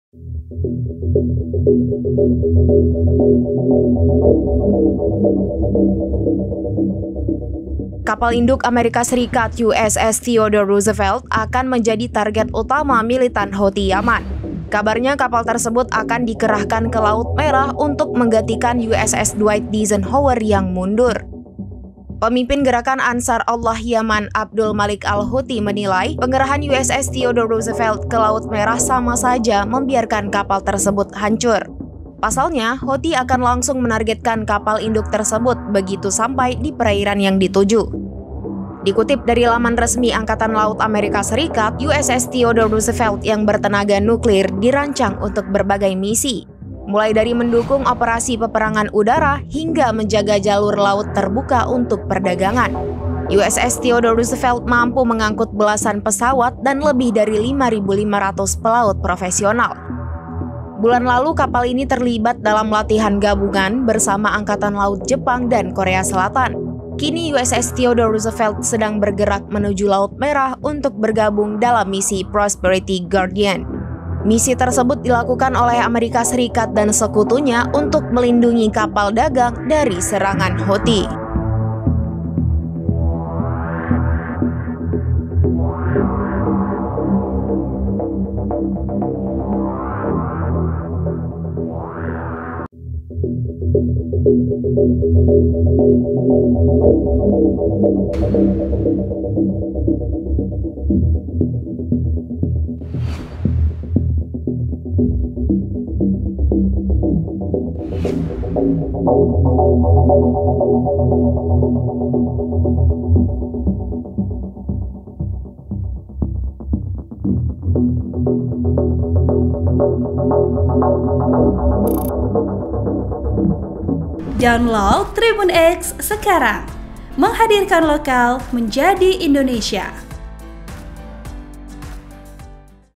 Jadi Target Houthi, Kapal Induk AS USS Theodore Nekat Lewati Laut Merah Usai Lawatan Negara
SERAMBINEWS.COM - Kapal induk Amerika Serikat (AS) USS Theodore Roosevelt disinyalir menjadi target utama kelompok Houthi Yaman.

Informasi menyebutkan kapal tersebut akan dikerahkan ke Laut Merah untuk menggantikan USS Dwight D.Eisenhower yang mundur.

Pemimpin gerakan Ansar Allah Yaman Abdul Malik Al-Houthi menilai, pengerahan USS Theodore Roosevelt ke Laut Merah sama saja membiarkan kapal tersebut hancur.

Pasalnya, Houthi akan langsung menargetkan kapal induk tersebut begitu sampai di perairan yang dituju.

"(USS Theodore Roosevelt) target utama bagi Pasukan Rudal Angkatan Darat Yaman mulai sekarang, dan akan menjadi sasaran begitu memasuki Laut Merah," kata Houthi, dikutip dari SputnikGlobe, Rabu (3/7/2024).

Dikutip dari laman resmi Angkatan Laut AS, USS Theodore Roosevelt yang bertenaga nuklir dirancang untuk berbagai misi.

Mulai dari mendukung operasi peperangan udara hingga menjaga jalur laut terbuka untuk perdagangan.

USS Theodore Roosevelt mampu mengangkut belasan pesawat dan lebih dari 5.500 pelaut profesional.

Bulan lalu, kapal ini terlibat dalam latihan gabungan bersama Angkatan Laut Jepang dan Korea Selatan.

Kini USS Theodore Roosevelt sedang bergerak menuju Laut Merah untuk bergabung dalam misi Prosperity Guardian.

Misi tersebut dilakukan oleh AS dan sekutunya untuk melindungi kapal dagang dari serangan Houthi.(*)

VO: Dara
Raja: Raja